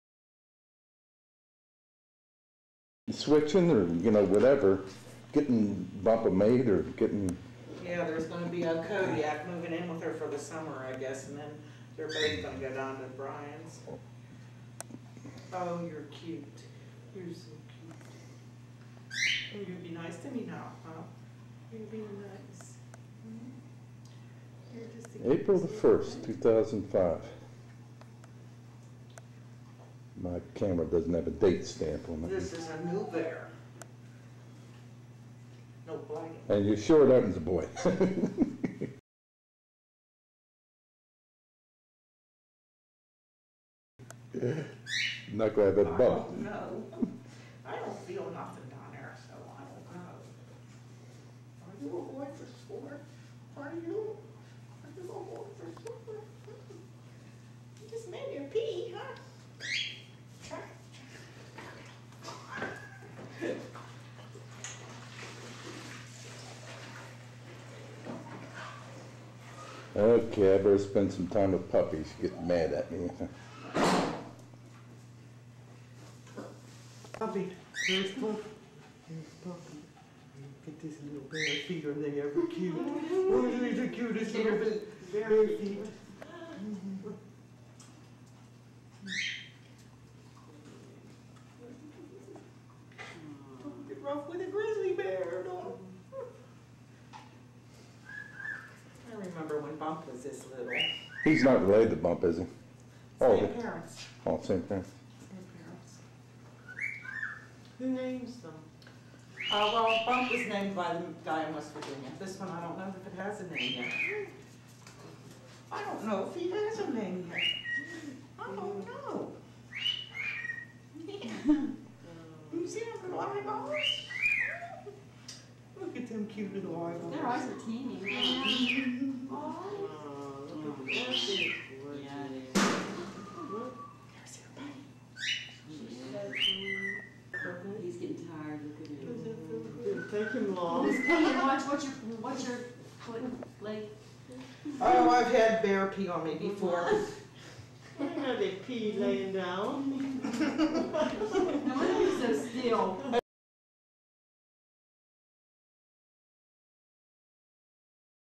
TIGER CUB & BEAR CUB - © Denmortube
Teeger and L'il Bear meet Zoo Volunteer Cheri for the first time.  Original video created by (c) Dennis Crossland at the former Woodland Zoo in Farmington, PA